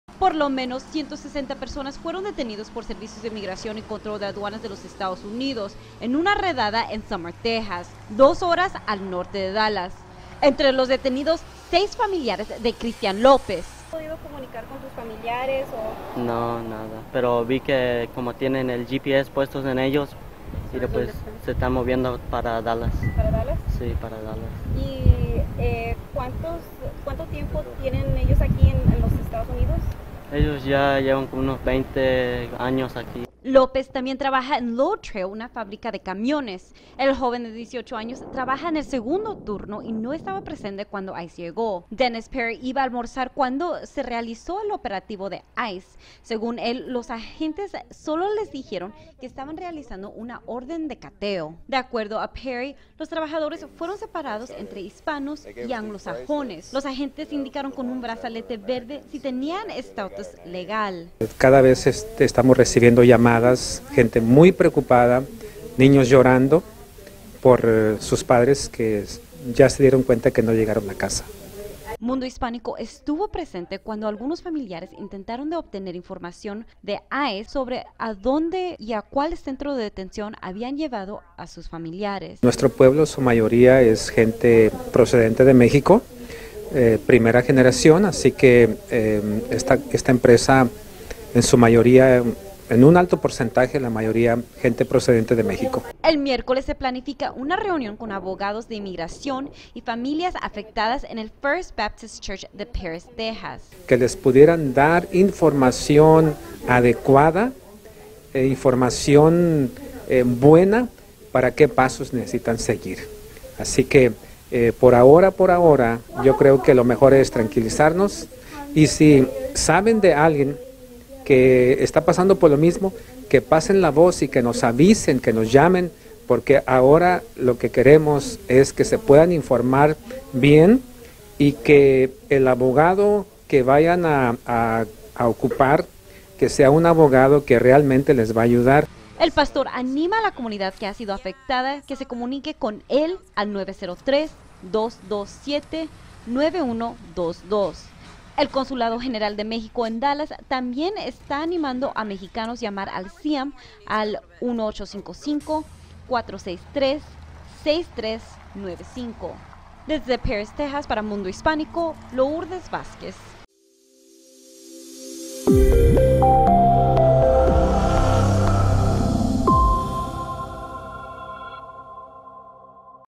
ICE confirma más de 100 arrestos tras masiva redada
Las autoridades federales confirmaron el martes por la tarde que se llevaron a cabo más de 100 arrestos tras una masiva redada de inmigración realizada en el norte de Texas.